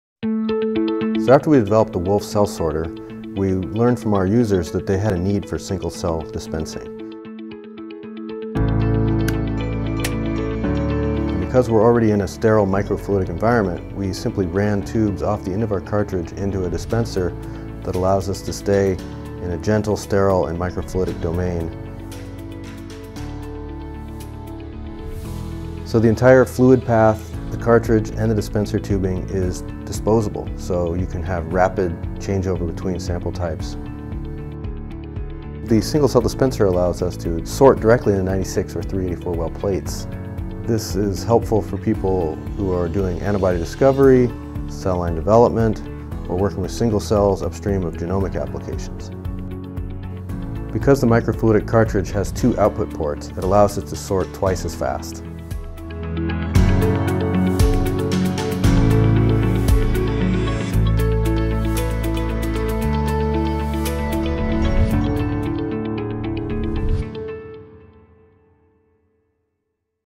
Nanocellect N1 Single Cell introduction
Many applications require the sorting and dispensing of single cells, such as B-cell identification in antibody discovery, and monoclonal CHO cell line development. Furthermore, single-cell ‘omics assays, where sample preparation is a key step for quality data generation, are no longer heroic academic exercises. Our dispensing method, like the WOLF®, remains aerosol-free, sterile, and disposable. Two outlets from the sorting cartridge are attached to tubing and two dispensing heads. Users can select the same cell population for every well, or assign two different cell populations. Each well of a 96- or 384-well plate can receive anywhere from 1 to 100 cells as they are sorted from the sample. Or users can simply dispense from a pure population. The N1 Single Cell Dispenser is seamlessly integrated into the intuitive software. Select single-cell from the dropdown menu, map out your plate, and hit go. The WOLF® and dispenser work together to deliver plated cells in a compact and affordable format. After sorting, data from each cell that are indexed well-bywell, allowing for confirmation of the plated cell.